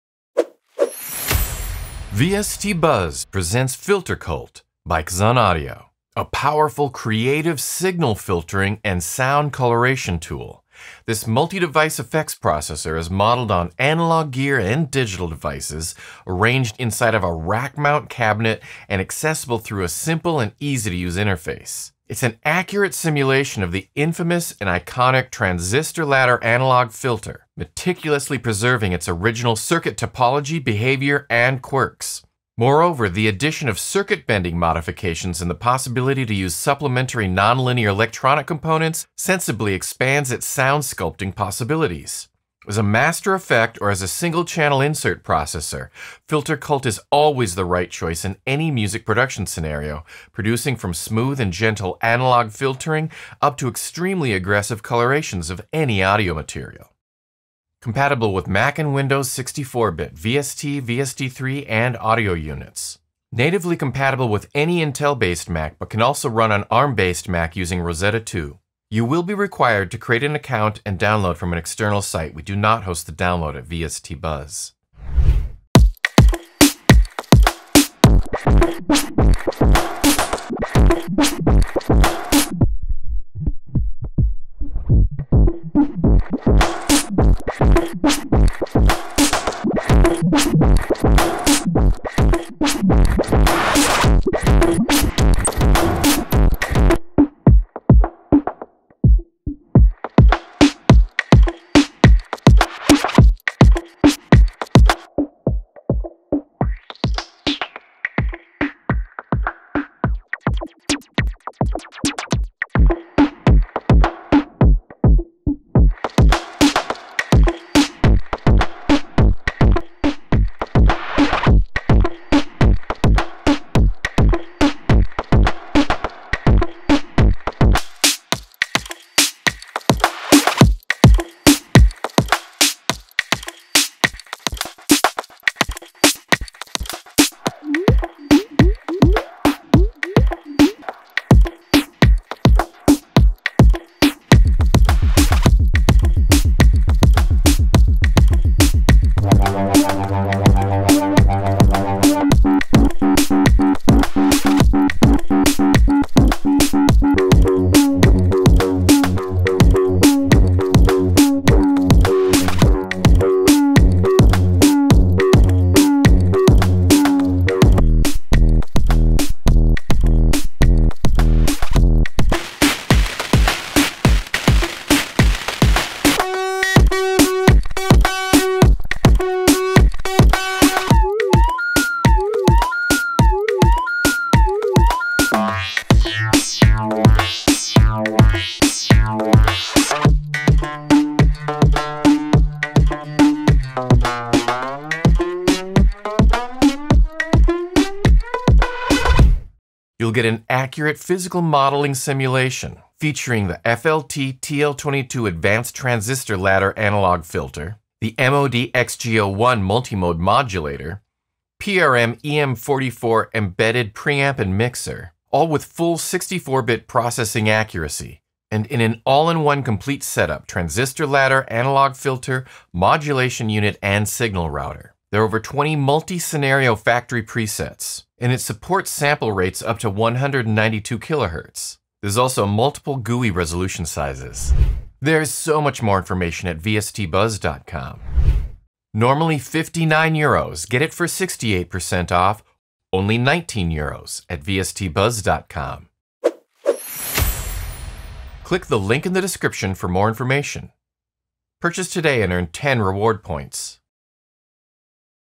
Xhun Audio FilterCult - 3 Min Walkthrough Video (68% off for a limited time)
0:00​​​​​​​​​​​​​​​​​ Capabilities & Requirements
1:14 Demonstration
3:18 Key Features

FilterCult is a powerful, creative signal filtering and sound coloration tool. This multi-device effects processor is composed of a set of modeled analogue gear and digital devices, arranged inside a rack-mount cabinet and accessible through a simple and easy to use interface.

As a master effect or as a single channel insert processor, FilterCult is always the right choice in any music production scenario, producing from smooth and gentle analogue filtering up to extremely aggressive colorations of any audio material.